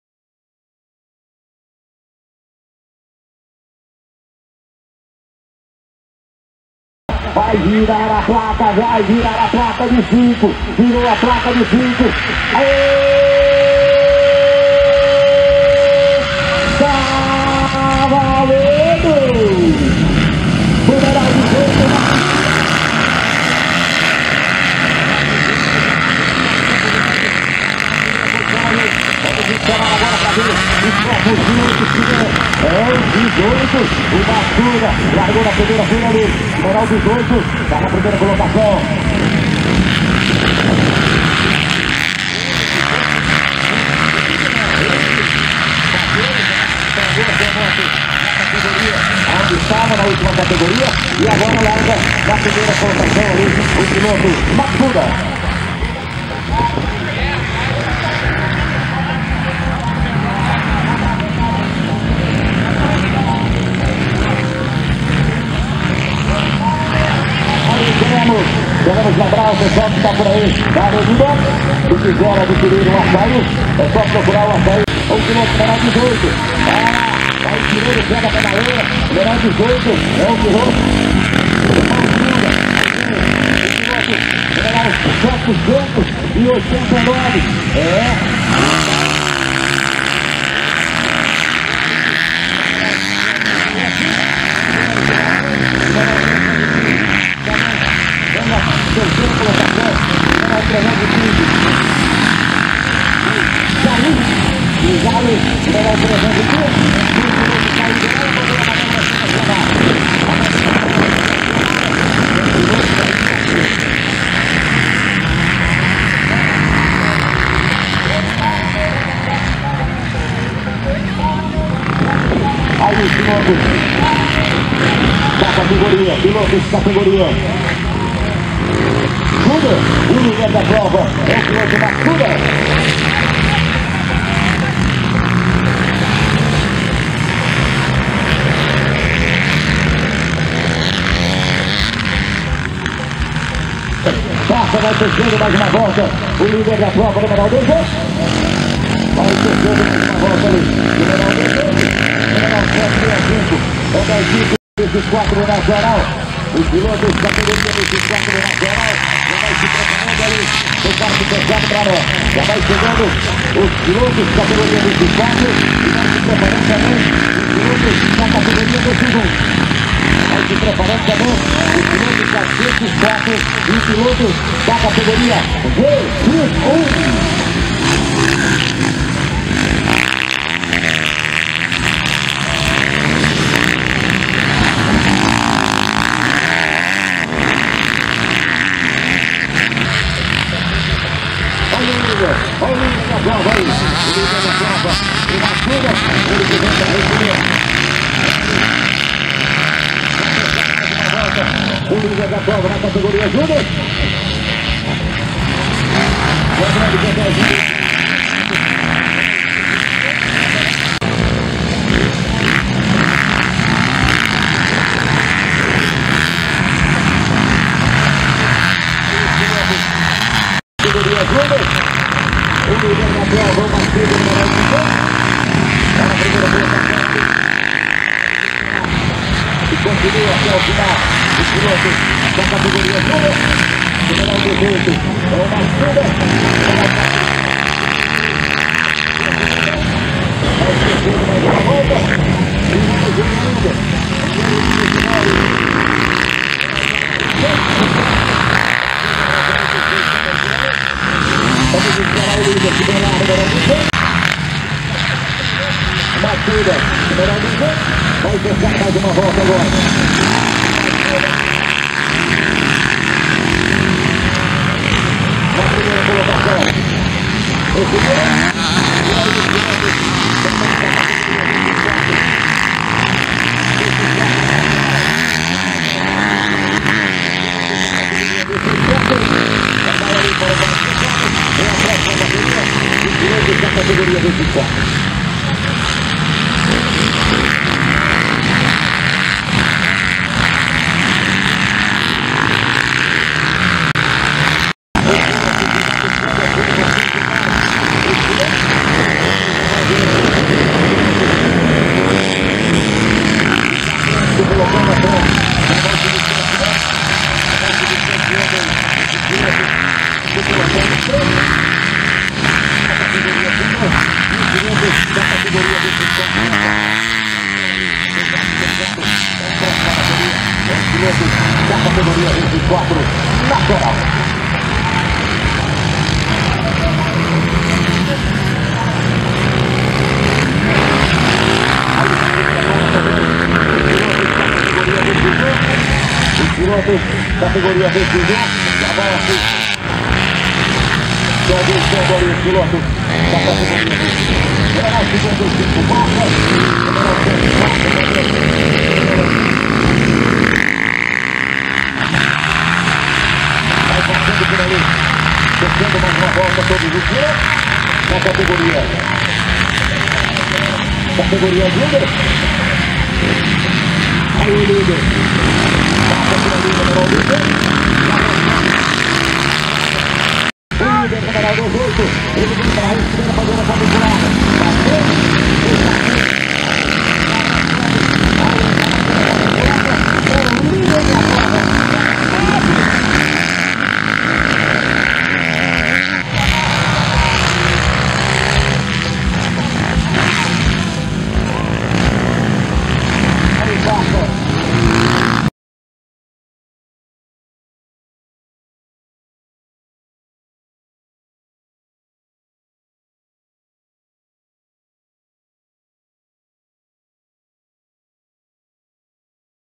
Acompanhe a vitória de Gustavo Matsuda na VX Junior em Sidrolândia
VX Junior
25/10 - Sidrolândia (MS) - Etapa 2
Copa Sul-mato-grossense de Velocross